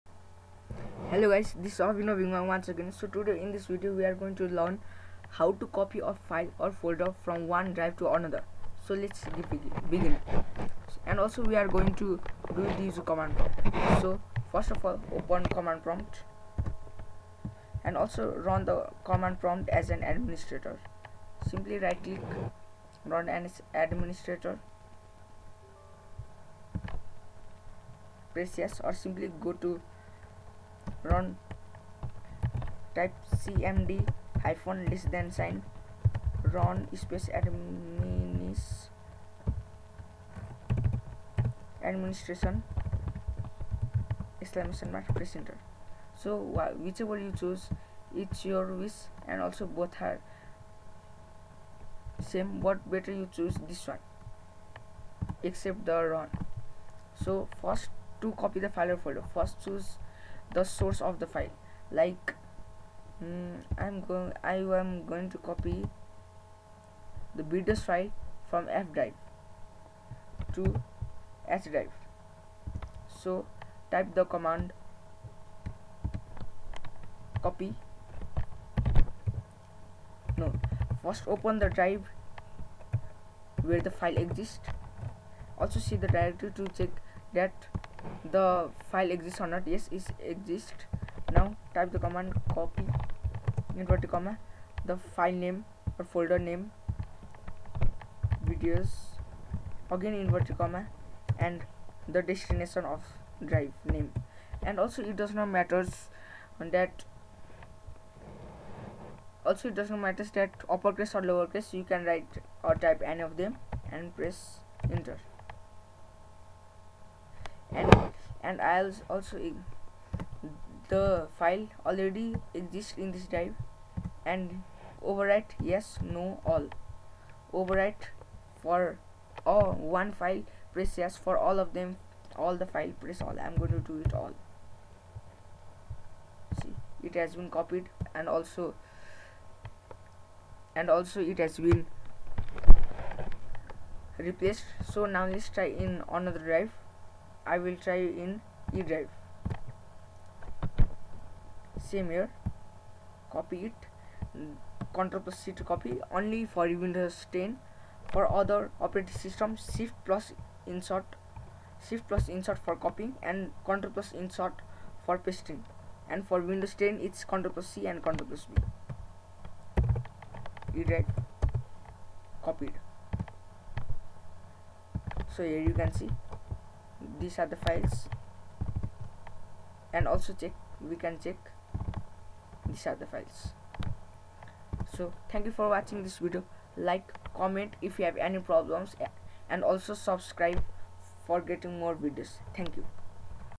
DIY Fastest Copying Method (Free/No Software Required)
Normally, when we copy massive data the speed of copying is affected by other background running programs too which eventually decreases your copying speed or decreases your background running application performances. Also, our CPU gets super hot which can be harmful to the processor and fan. So, going a little classic and archaic, we use simple command lines to copy such massive data from one point to another 10 faster which does not have any adverse effects and your CPU to remains safe!

Important note: Run command prompt as an "administrator"

command:

[source drive]: (to open the source drive)
copy "file name" destination drive
    OR
copy "full location of source file" destination drive/folder